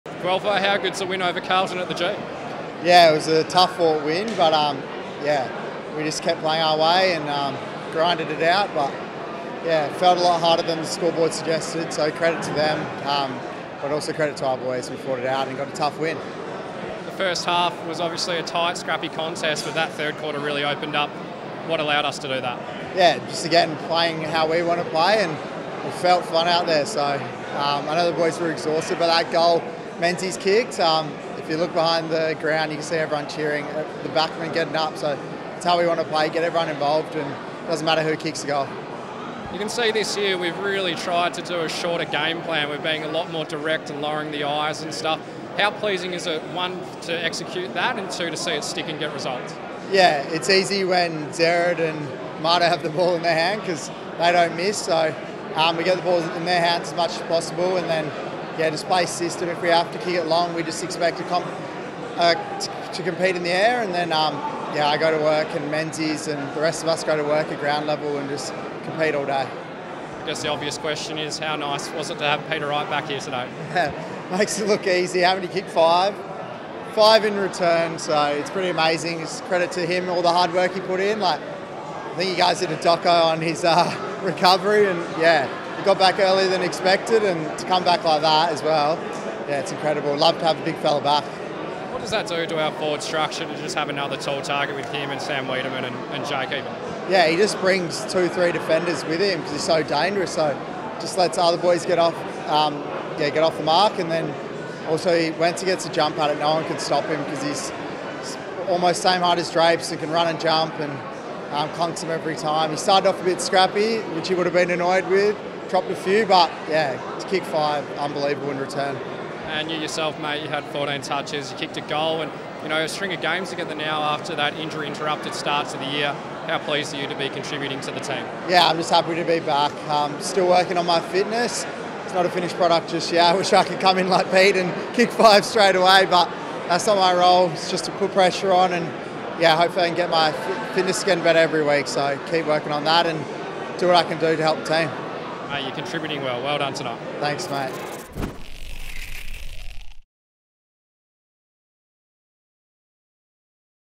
Matt Guelfi | Post Game Interview | Round 13 | 2023
Matt Guelfi chats following our round 13 win over the Blues at the 'G